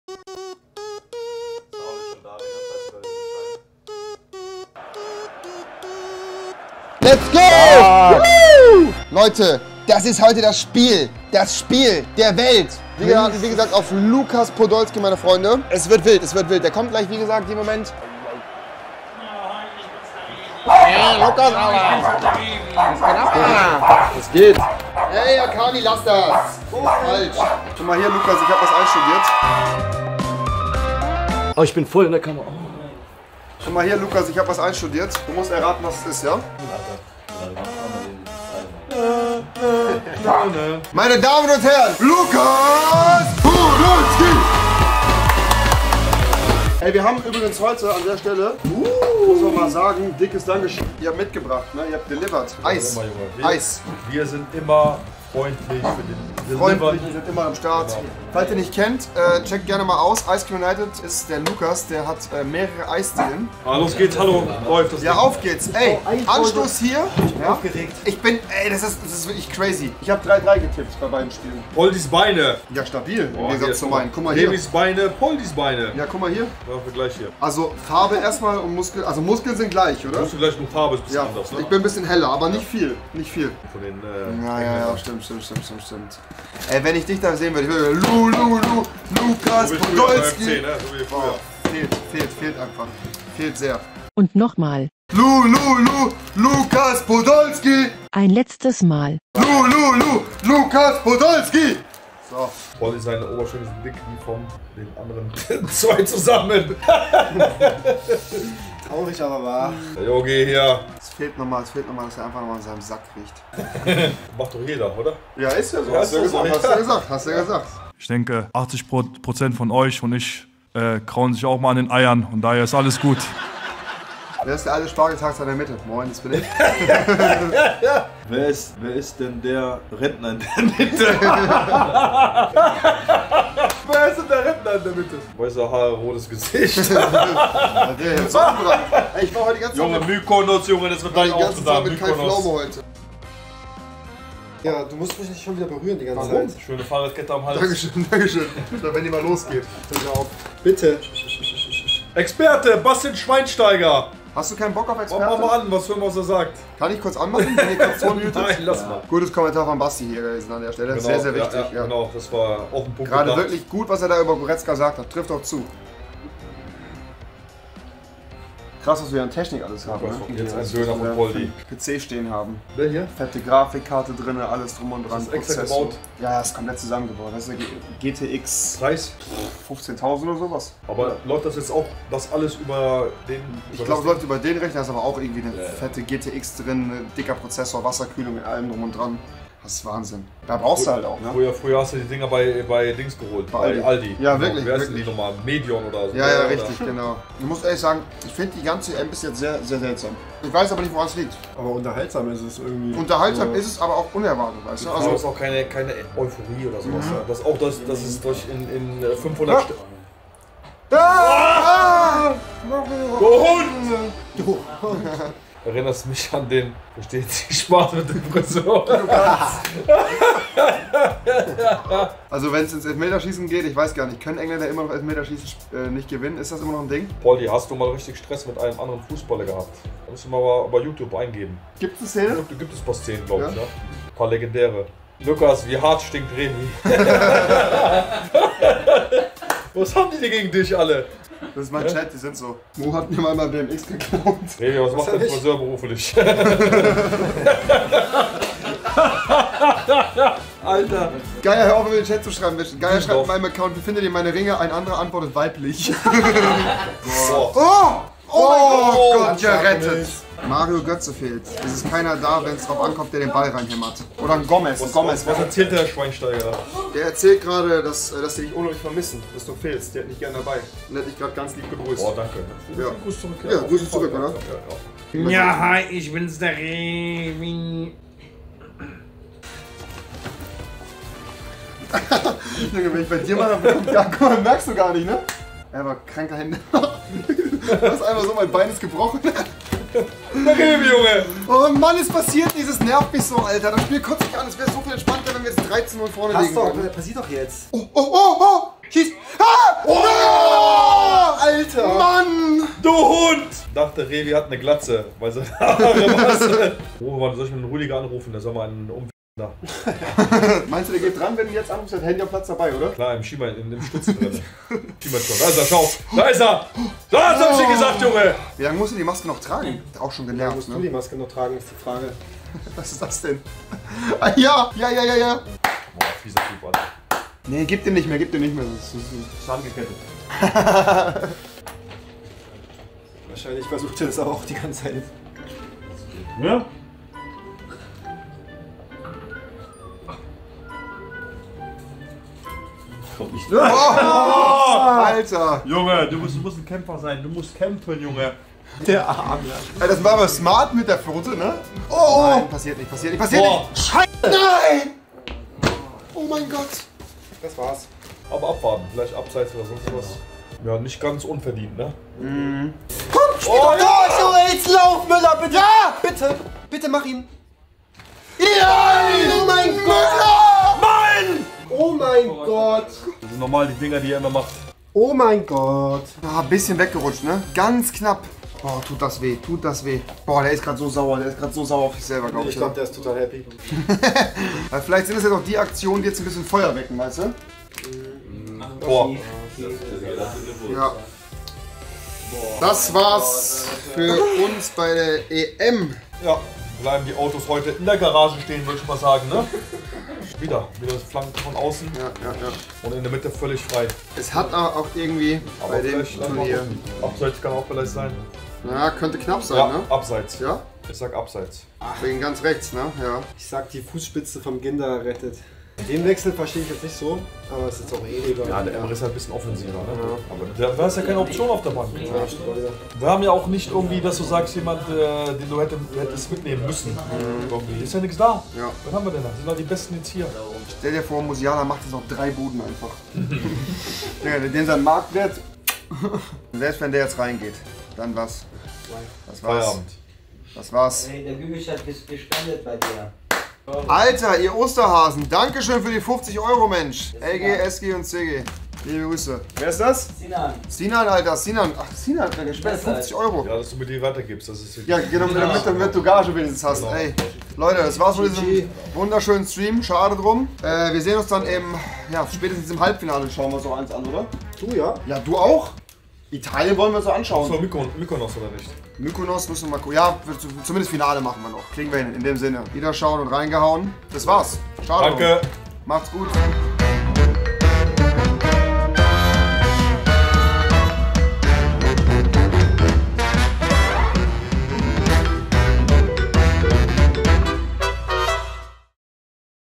DE 🇩🇪 - ENG 🏴󠁧󠁢󠁥󠁮󠁧󠁿, aber Lukas Podolski mobbt mich bisschen..
Es war wieder EM... Deutschland gegen England... wie es ausgegangen ist wisst Ihr ja alle 😞
Trotzdem hatten wir gestern einen sehr lustigen Stream zusammen mit @MOOO__ und unserem Star Gast, Lukas Podolski!
Hat echt mega Spaß gemacht, wen könnten wir für das Halbfinale und das Finale einladenß Schreibts gerne in die Kommentare! 🤫